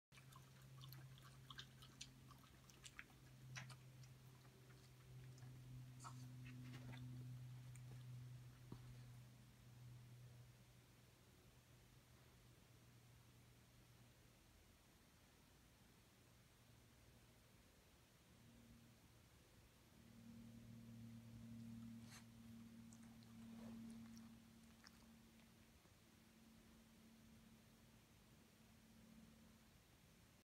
Cali’s thirsty
Cali getting her drink on, she thinks she’s people.